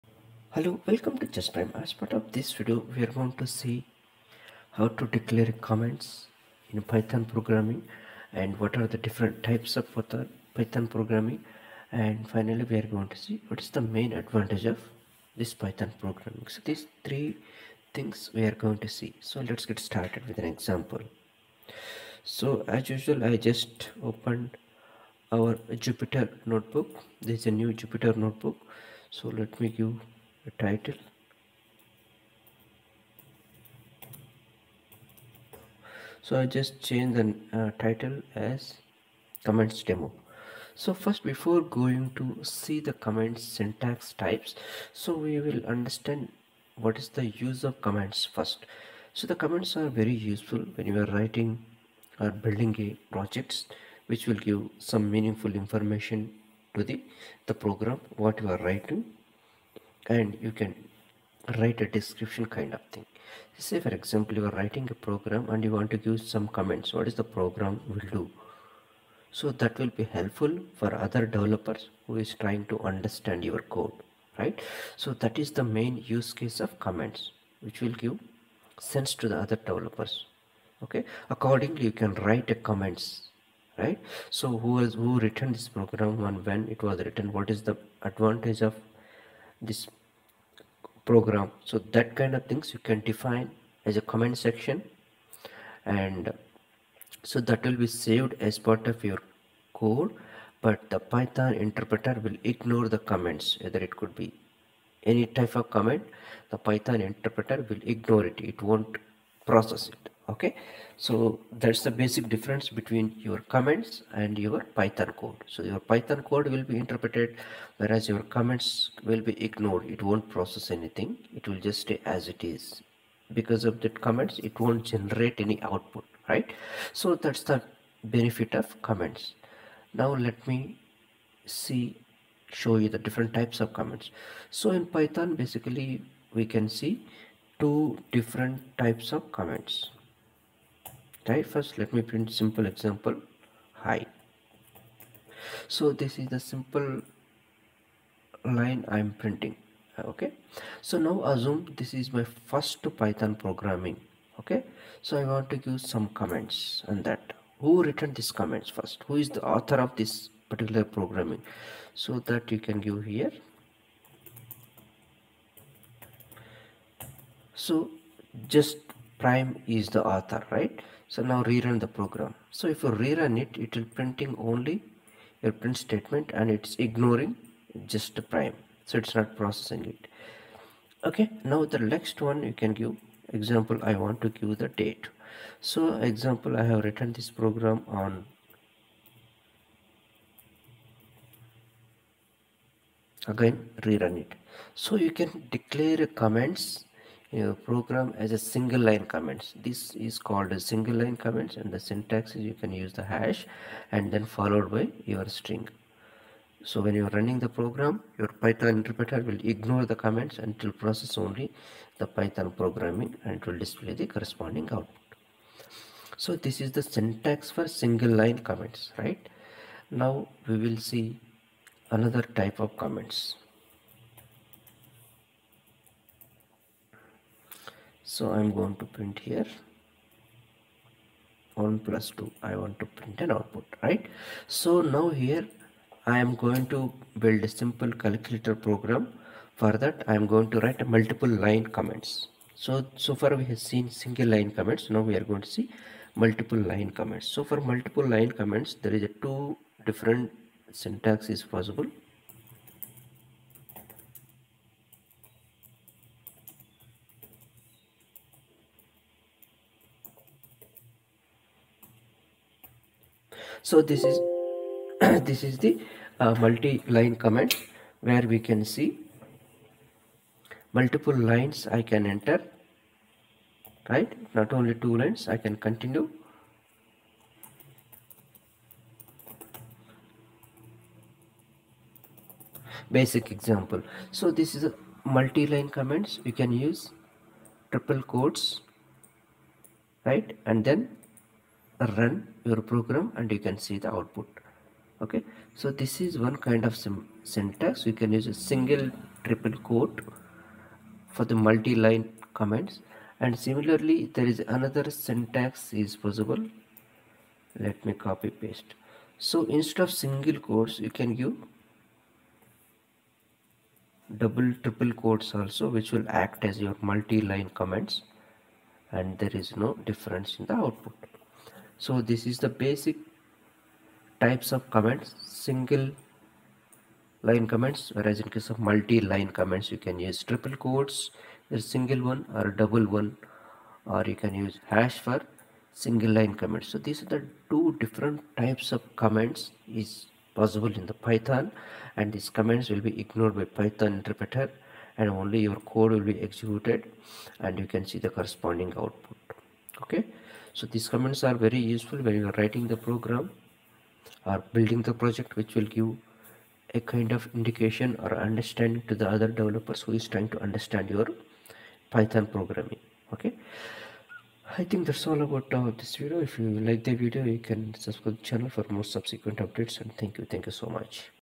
5.Comments in python | Python Tutorial
In this video , we are going to learn what are the comments in Python .
how to put comments in python script ,
what is use of comments in python and much more things about comments in python in detail.
Types of comments in python and their usage.
1.Single line comments
2.Multi line comments